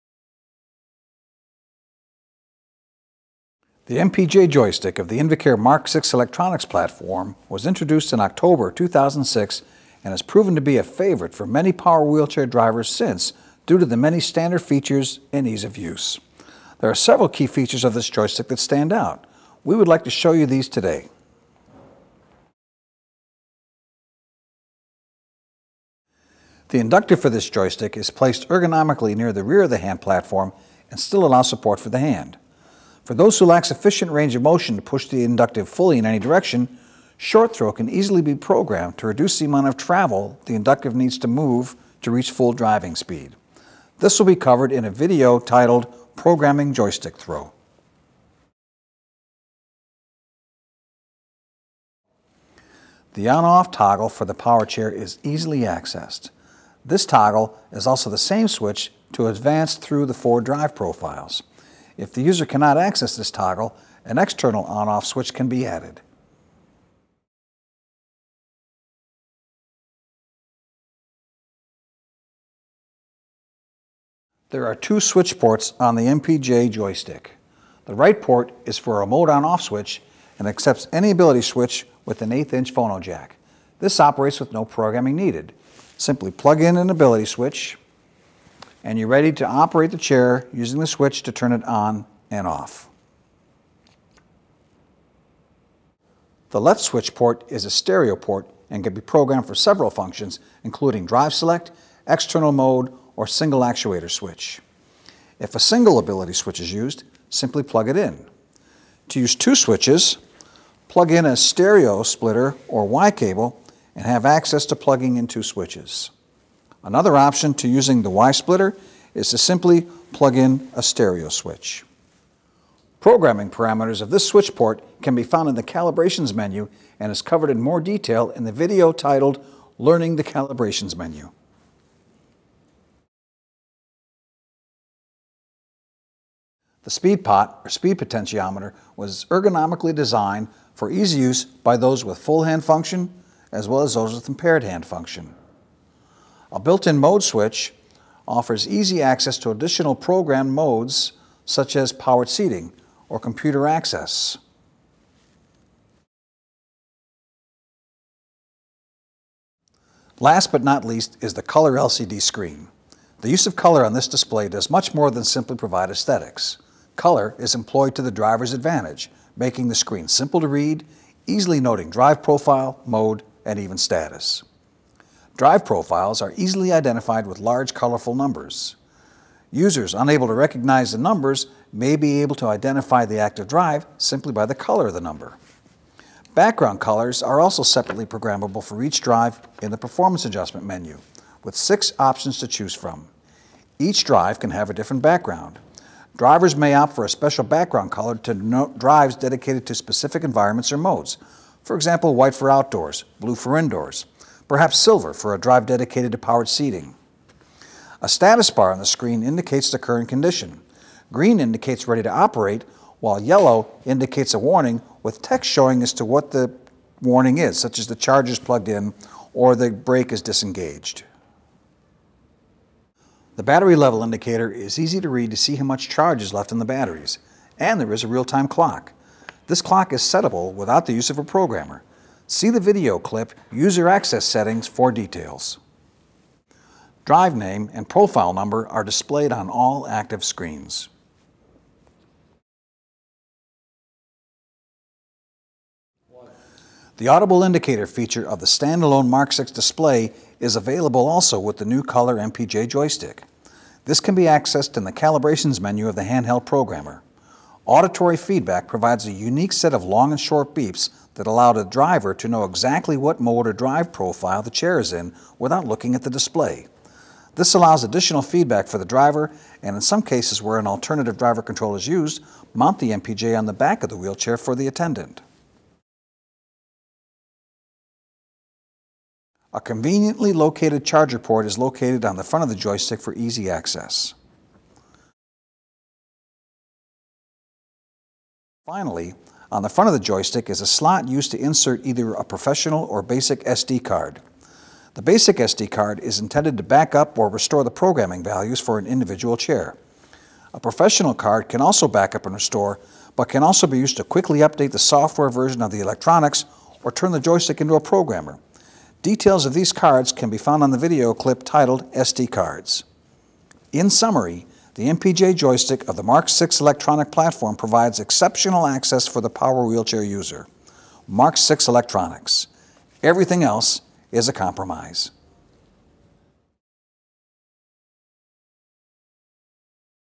MPJ+ Joystick with Color Display
Get an overview of all that is possible with the MPJ+ Joystick with Color Display through this Invacare MK6i Electronics Virtual Tutor.